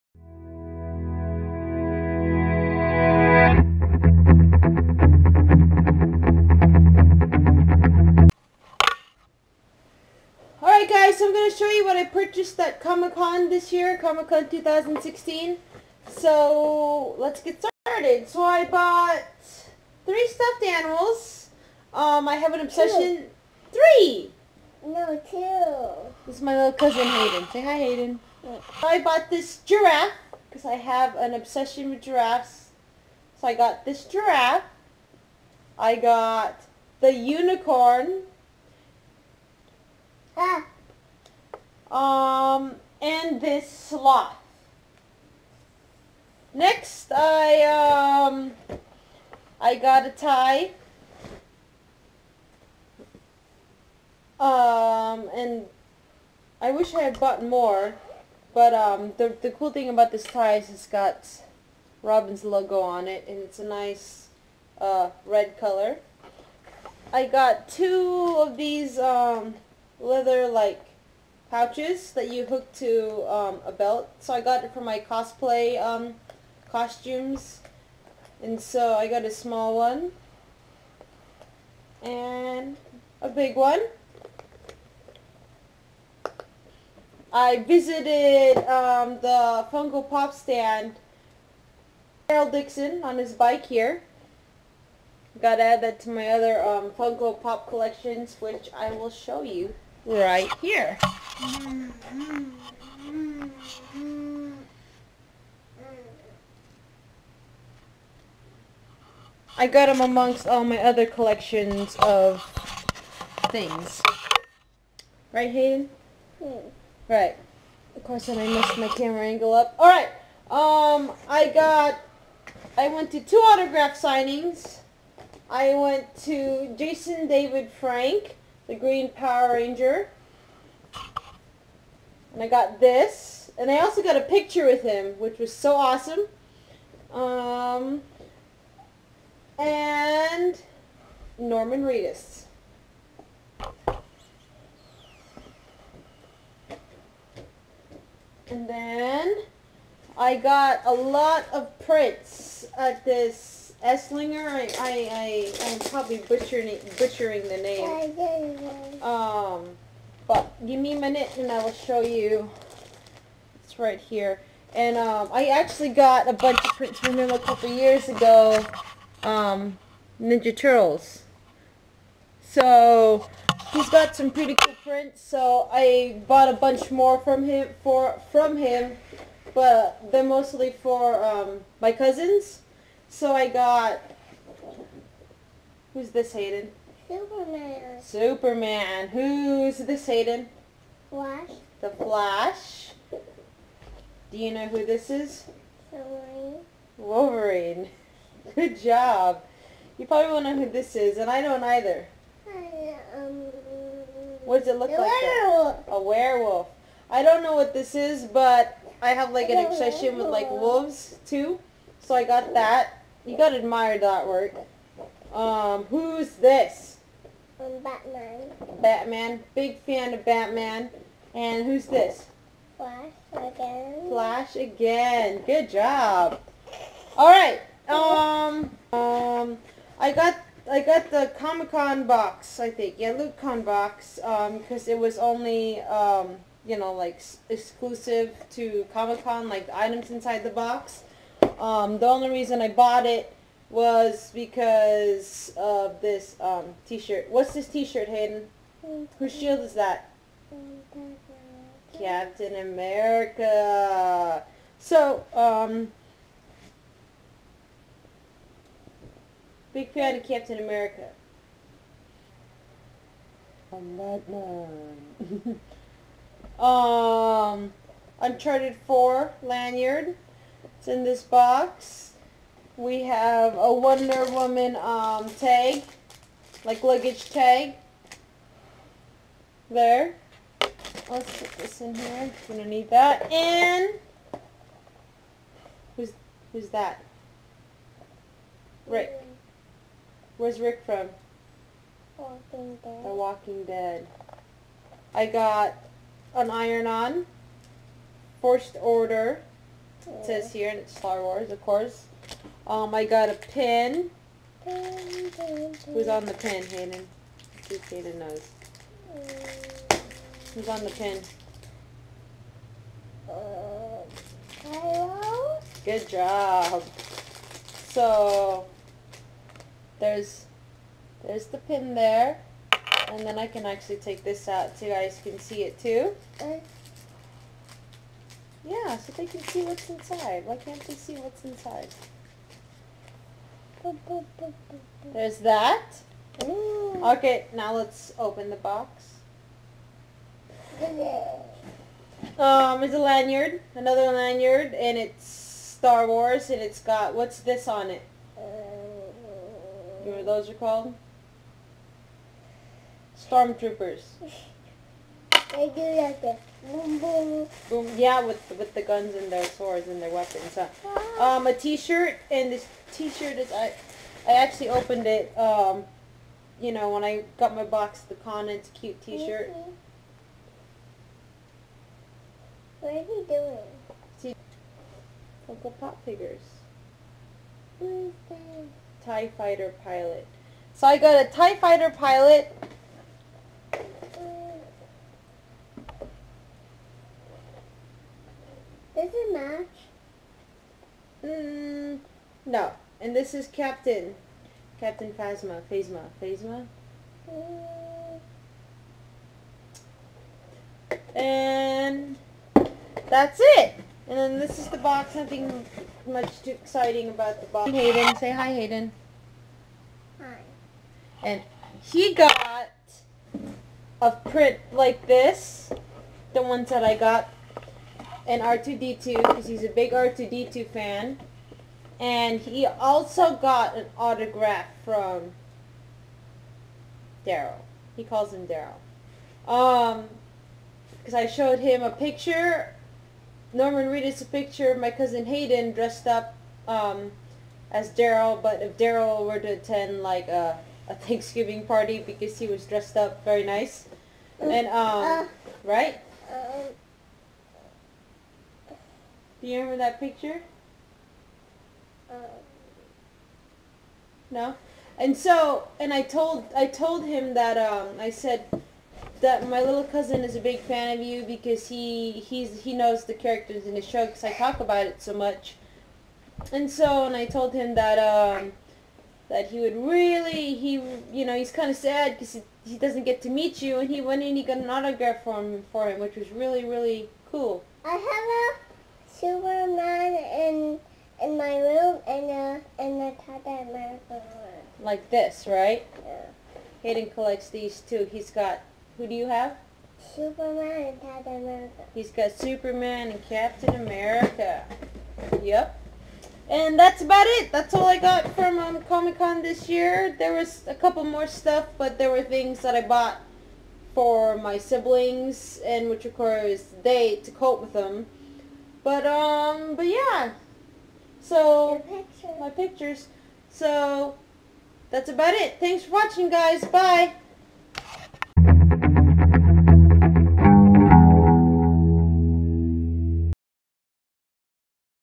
Spoils from Emerald City Comicon
A little late, way late on the uptake.
But here is what I bought at ECCC 2016.
Feat. My Cousin Hayden